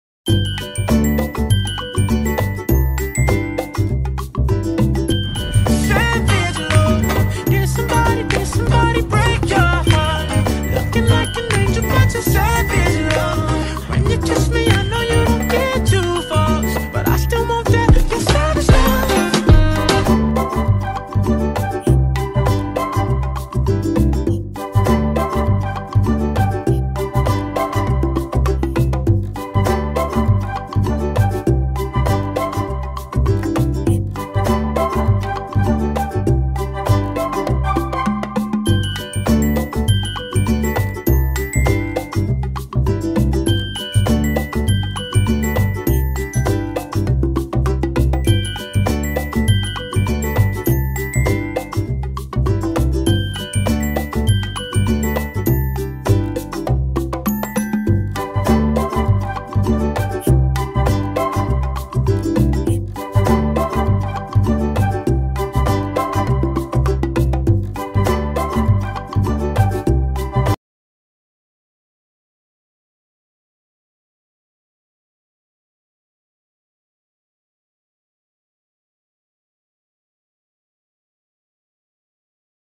GUCCI 3 PCS SET
High quality Gucci 3 PC set with long belt and stylish look